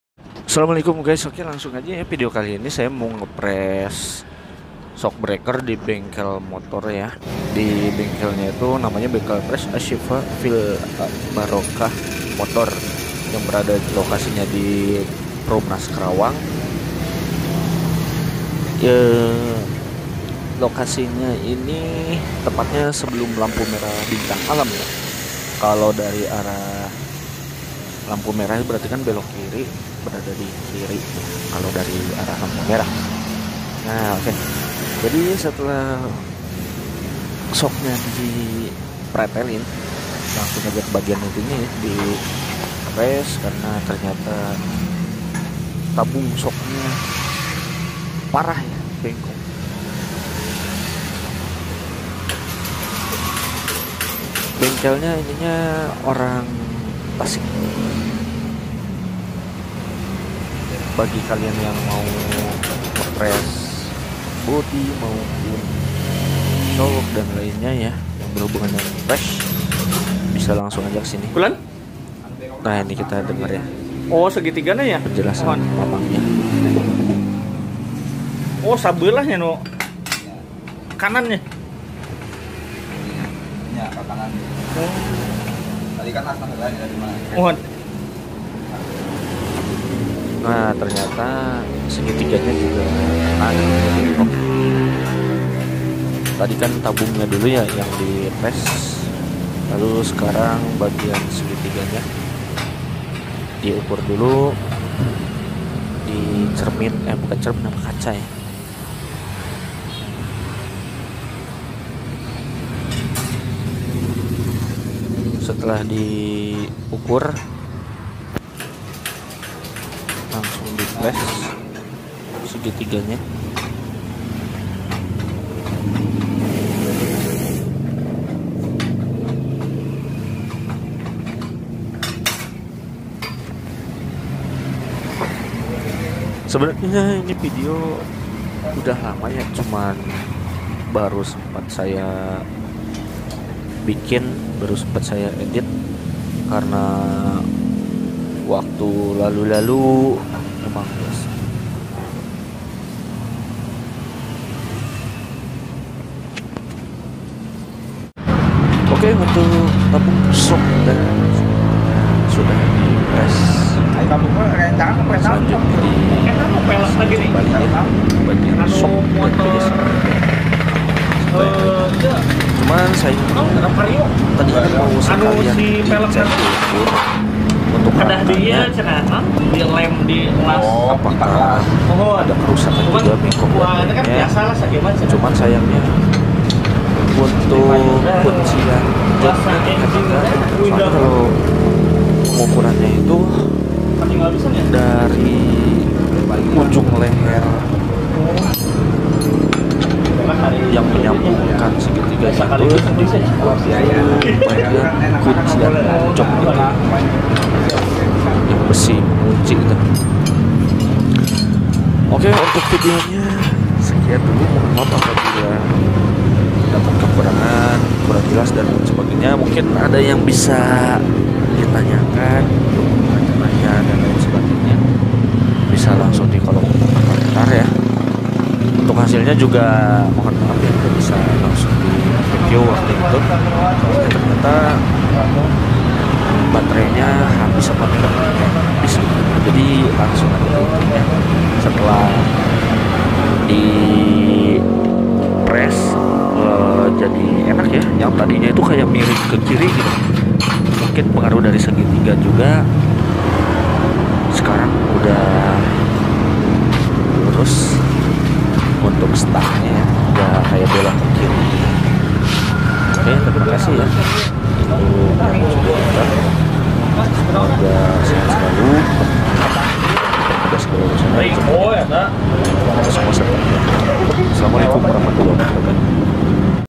Press Segitiga & Shock Satria FU
Video Dailly Vlog Satria FU.
VIDEO INI BUKAN TUTORIAL ya guys.
Video kali ini tentang Repair Segitiga & Shock Satria FU.
Video ini dishoot sebenarnya udah lama, tapi baru sempat edit dan upload sekarang.